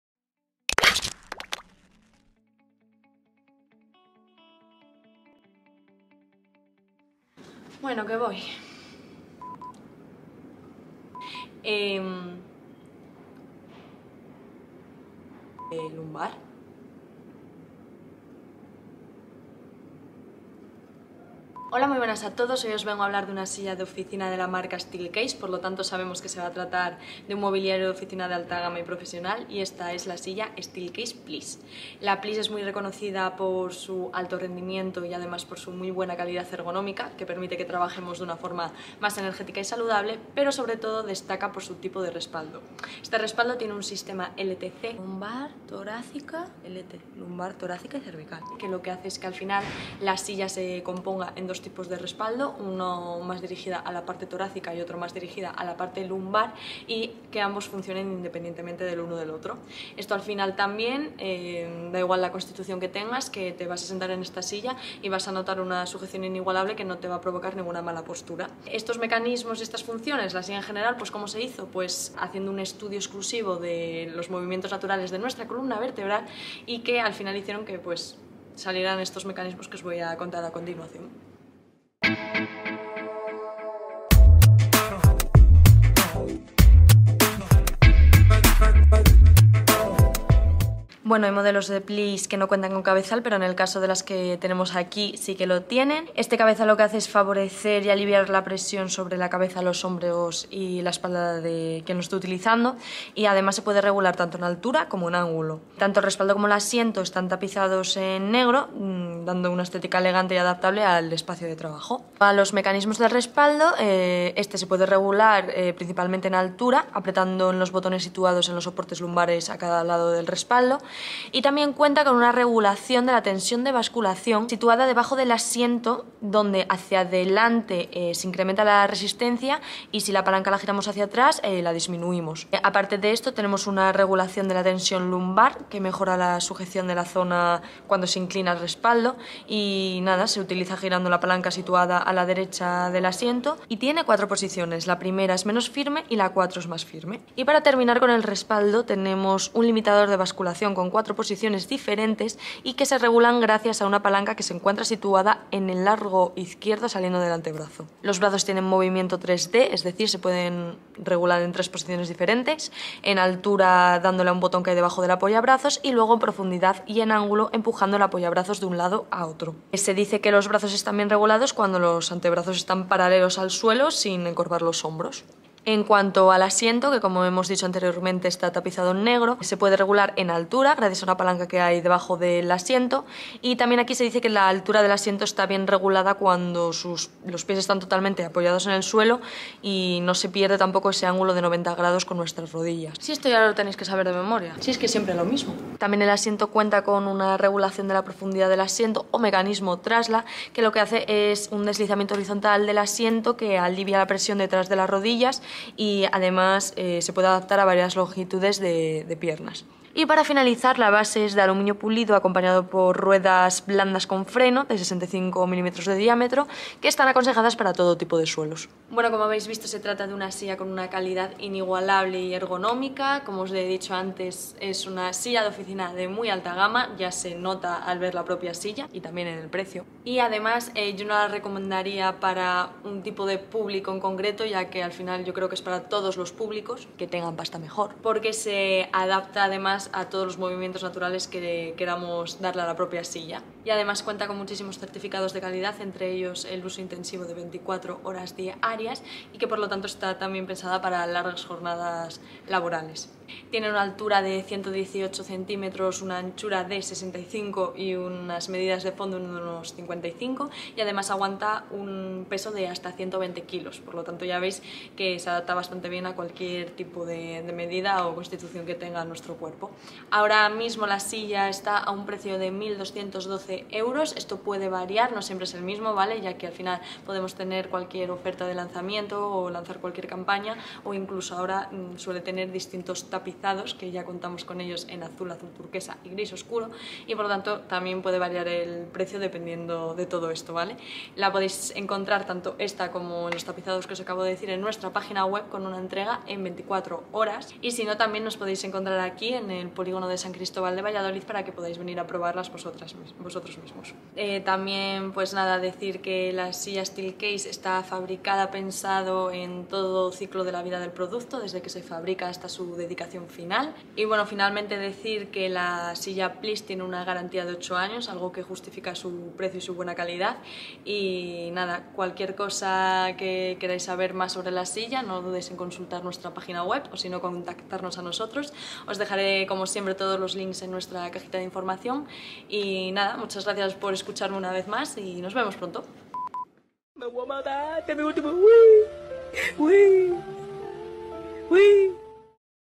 Análisis y mecanismos de la silla STEELCASE PLEASE II | La mejor silla de alto rendimiento.
Os traemos una de las mejores sillas de la marca de alta gama Steelcase. La silla Please destaca muy su alto rendimiento y su sistema LTC (Lumbar, Torácico y Cervical) compuesto por dos tipos de respaldo adaptado a todo tipo de cuerpos  y dando un mayor confort a la columna vertebral.

Disponible desde un precio de 1212€ en color negro y contando por primera vez con más tapizados. Garantía de ocho años y entrega en 24 horas.

Aquí podéis ver modelos, fichas técnicas y mas detalles de la STEELCASE PLEASE II en cualquiera de sus versiones.